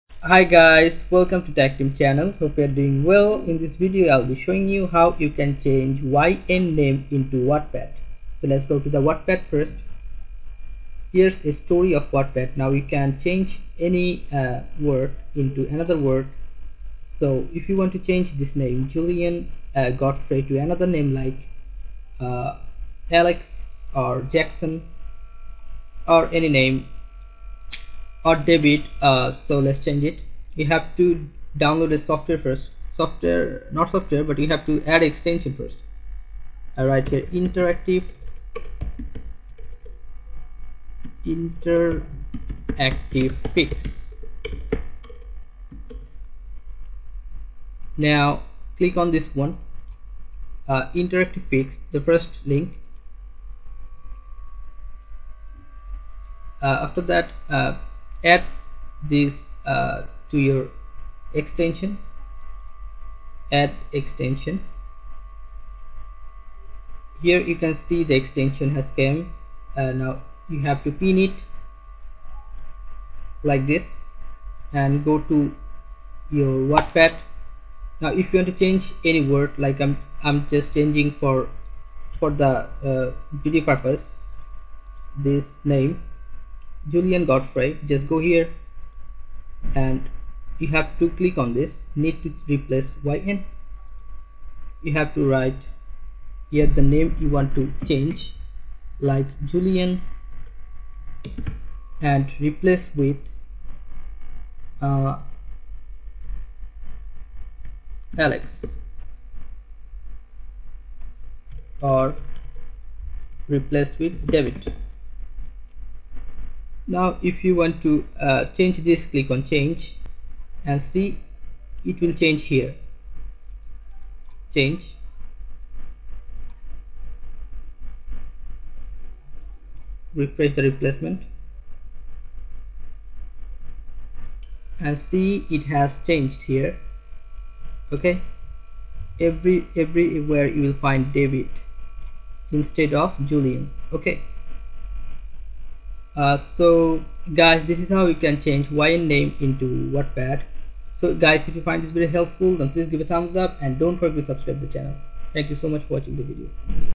How to Change "Y/N" to Your Name on Wattpad | Customize Your Reading Experience 2025
Are you tired of seeing "Y/N" in your favorite Wattpad stories? Wish you could replace it with your own name and immerse yourself fully in the narrative? Look no further! In this tutorial, we'll show you step-by-step how to change "Y/N" to your own name on Wattpad.

Wattpad is a popular online platform where millions of writers and readers connect, but sometimes the generic placeholders like "Y/N" can break the immersion. Thankfully, with a few simple tricks, you can personalize your reading experience and make the story feel more personalized.

In this video, we'll guide you through the process of changing "Y/N" to your name on Wattpad. We'll cover various methods, including browser extensions, custom scripts, and other techniques that work across different devices. Whether you're using Wattpad on your computer, phone, or tablet, we've got you covered.

So, if you're ready to dive into the story and see your name instead of "Y/N," this tutorial is for you! Watch the video and start customizing your Wattpad experience today.